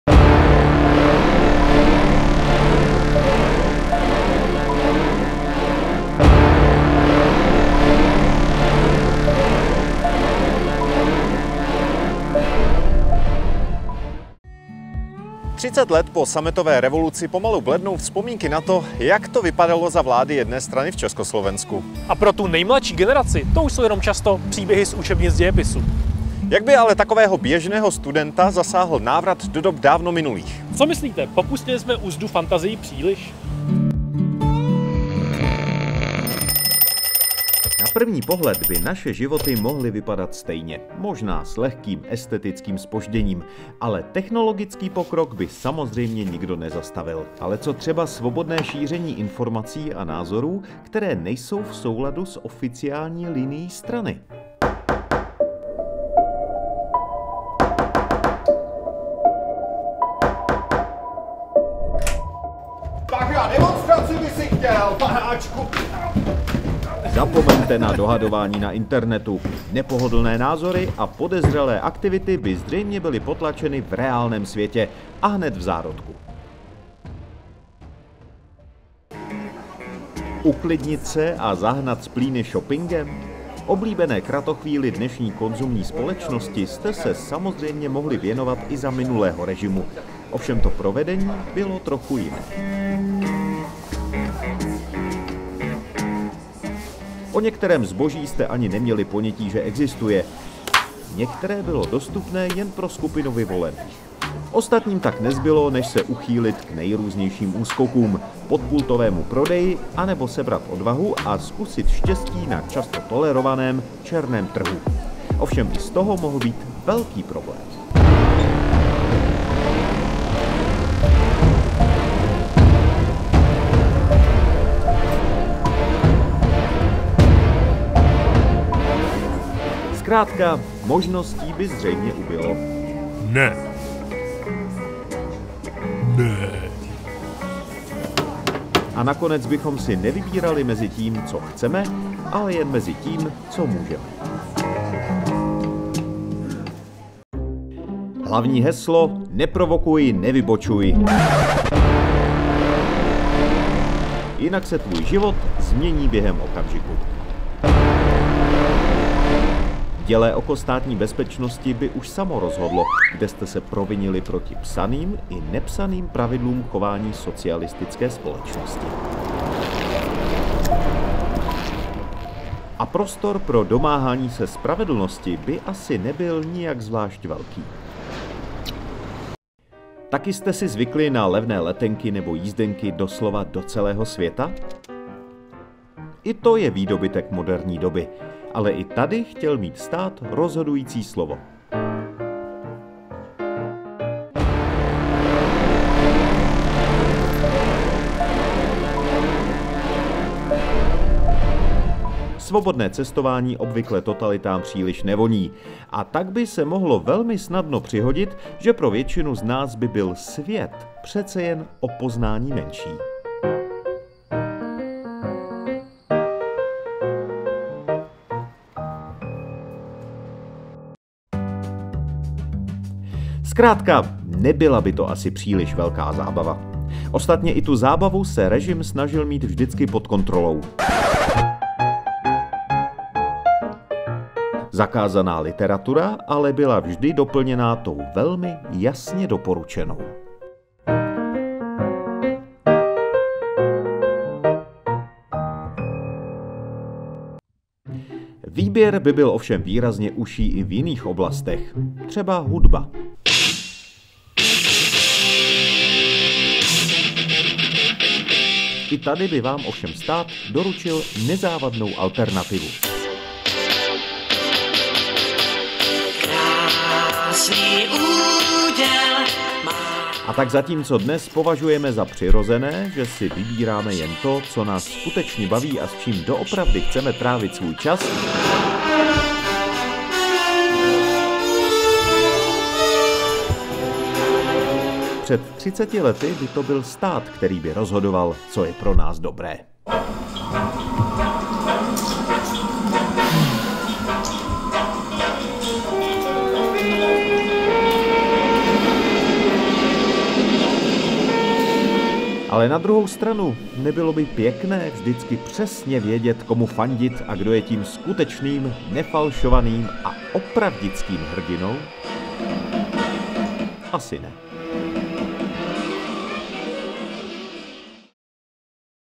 Jak by dnes vypadalo Česko bez listopadu 1989?
Vyhazov ze školy, zákaz cestování nebo rovnou pár ran obuškem? Pro dnešního studenta jsou represe kvůli nepohodlným názorům už nepředstavitelné. Jak by ale takový mladík dopadl, kdybychom ho přenesli o více než 30 let nazpět?